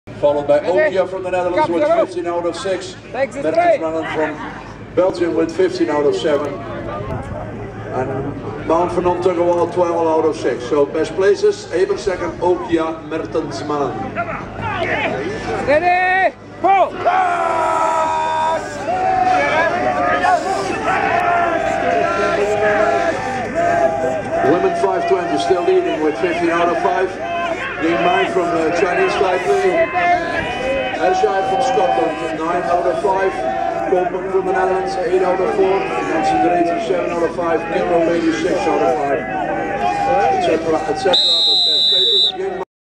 TTV Vandakker vs Waltenschwil-Kallern - End 1
First pull TTV Vandakker against Waltenschwil-Kallern

Open club European Championship Belfast 2015